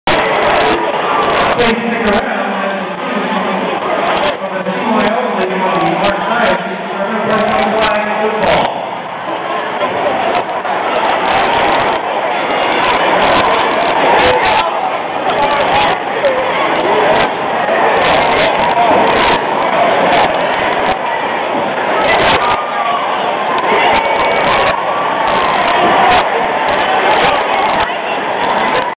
Marion Mayhem
Taken May 3, 2008

Before a game against the Lehigh Valley Outlawz.  The Mayhem fell short 33-43.